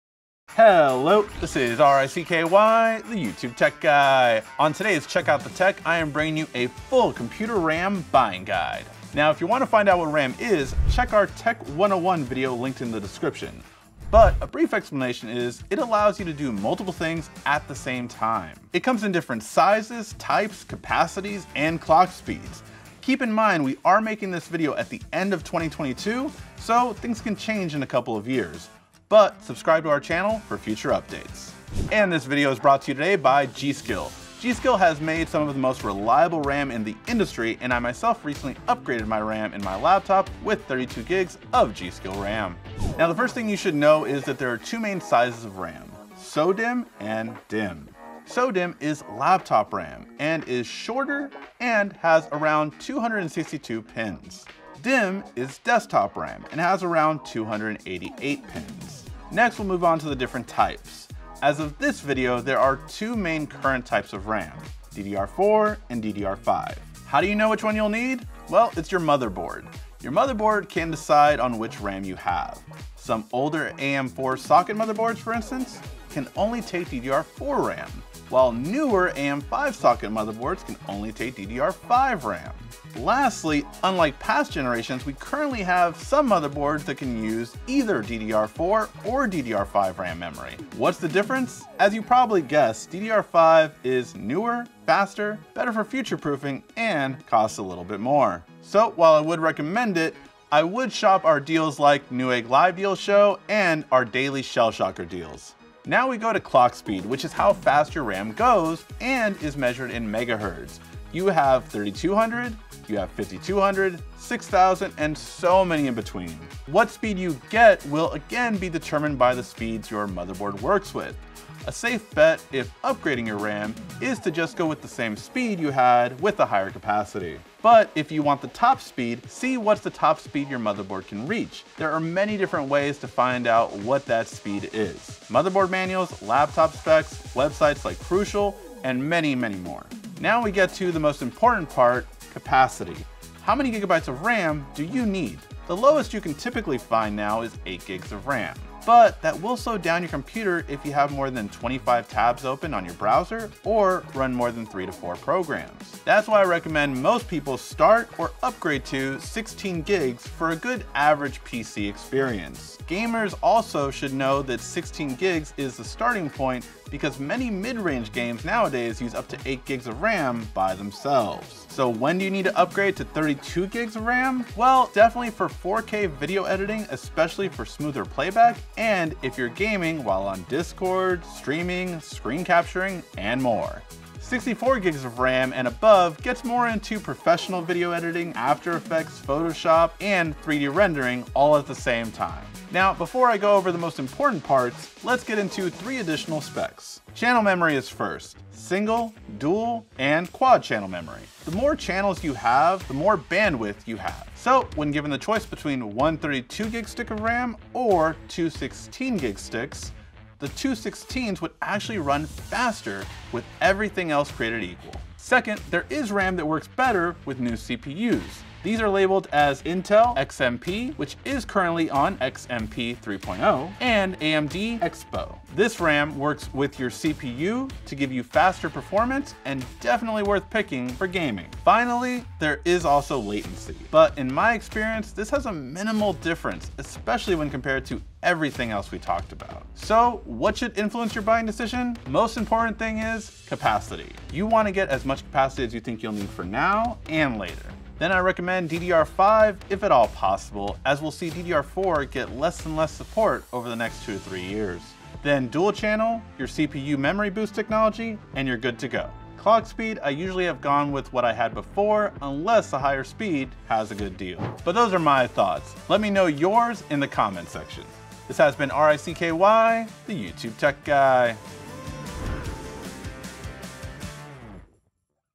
Computer RAM Buying Guide 2022 - Check Out The Tech
Upgrading your RAM? How much do you need? DDR4 vs DDR5? Which RAM do you have in your computer right now?